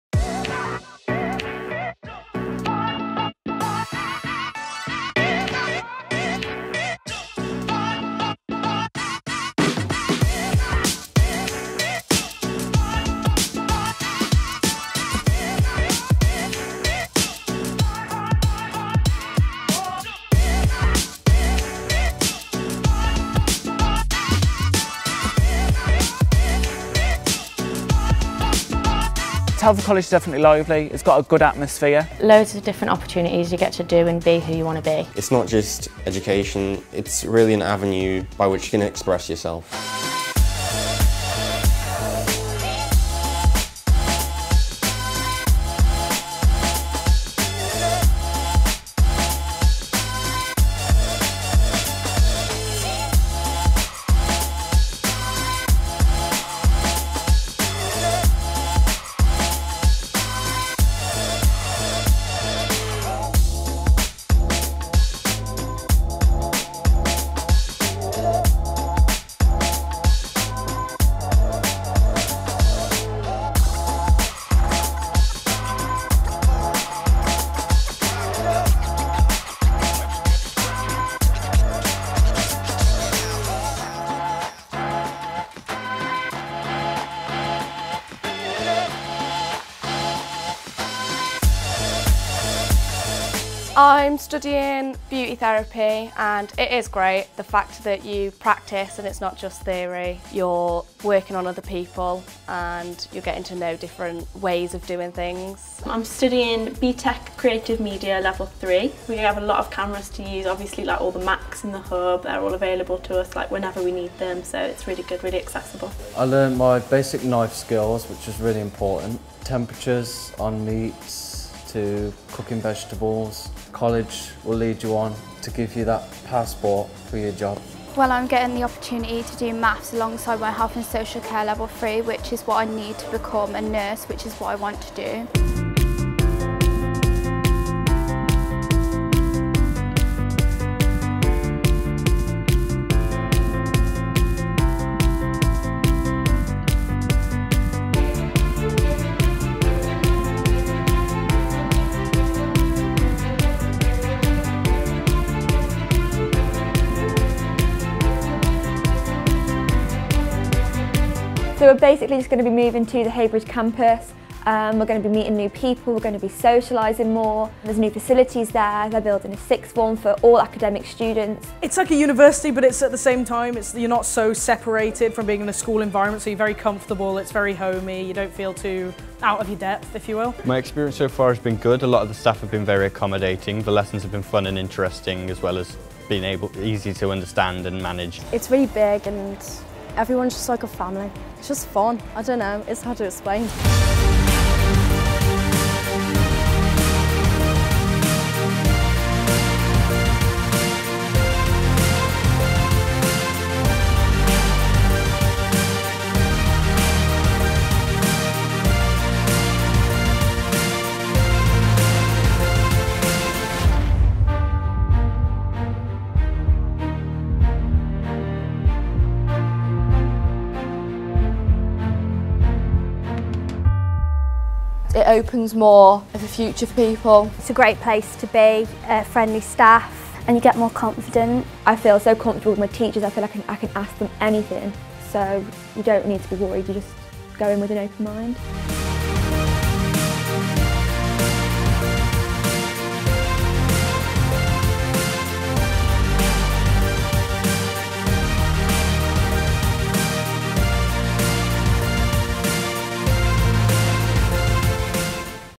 Define your future at Telford College
Define your future with Telford College,  you can build your career whether through A Levels, vocational studies, apprenticeships or directly into higher education. Telford College is the place for you.